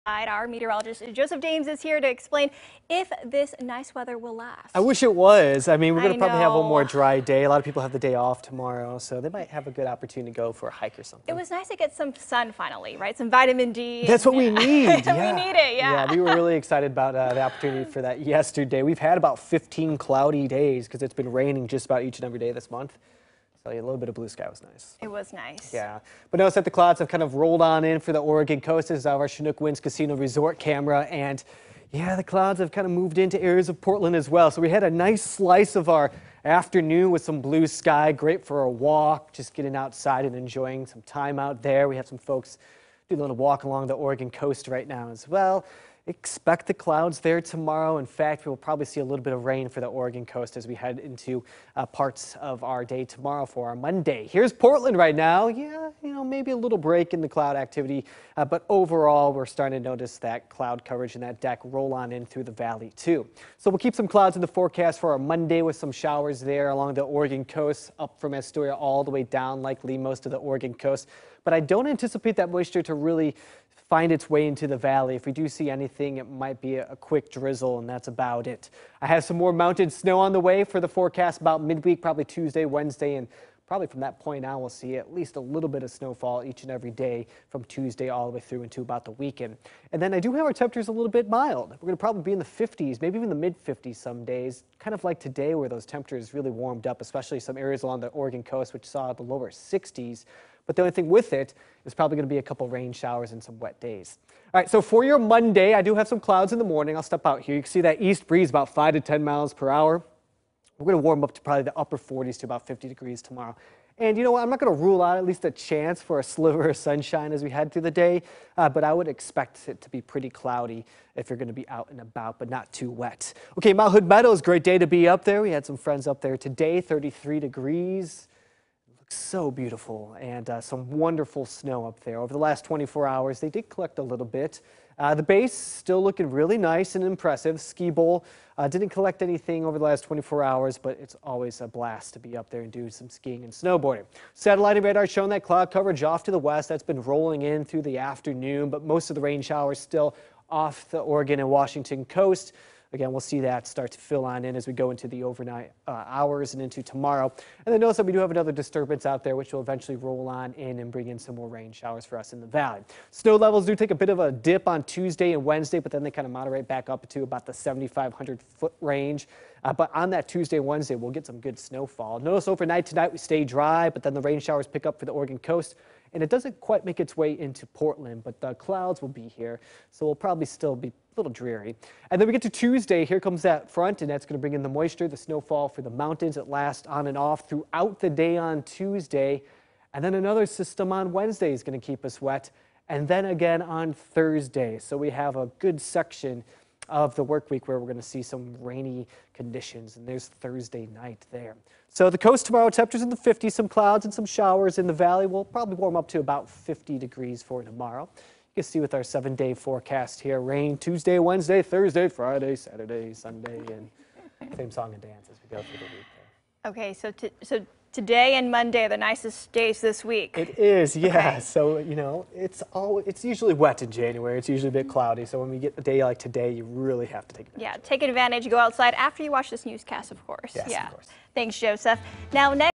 Weather Forecast: Above average temperatures with rain this week in Portland
Joseph with your weather update